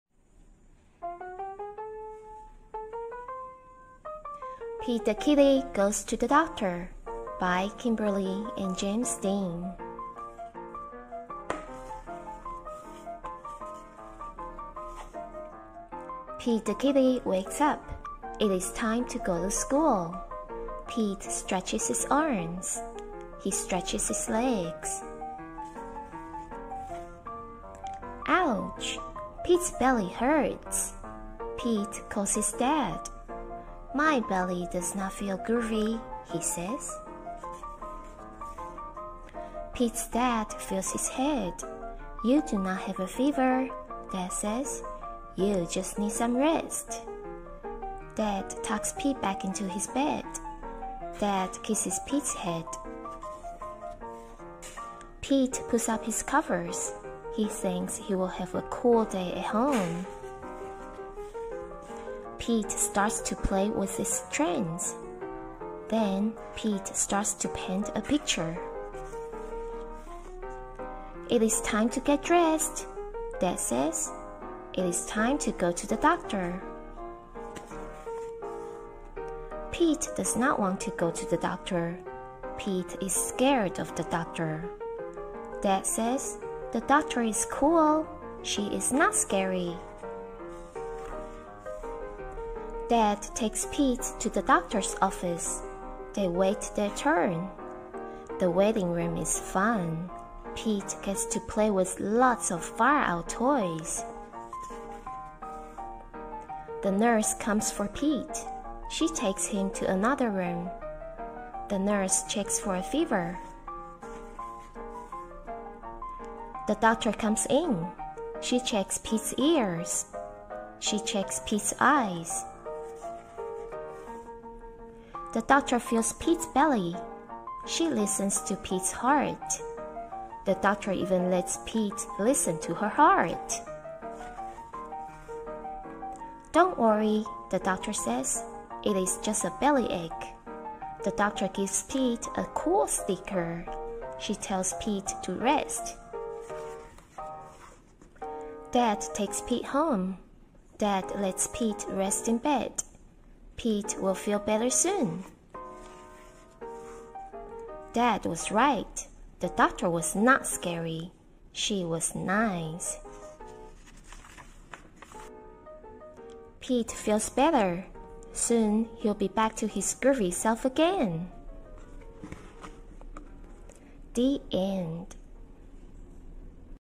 Pete the Kitty GOES TO THE DOCTOR by Kimberly & James Dean
Oh no! Pete the Kitty has a tummy ache. Good thing his dad knows just what to do. It’s time to go to the doctor!

Except Pete may be a teensy-weensy bit nervous about his first visit to the doctor. With the help of his dad, Pete discovers the doctor isn’t scary.

Beginning readers will easily relate to Pete the Kitty as he overcomes his fear of going to the doctor's office.

( I am not a professional reader, but I hope you enjoy reading with me.)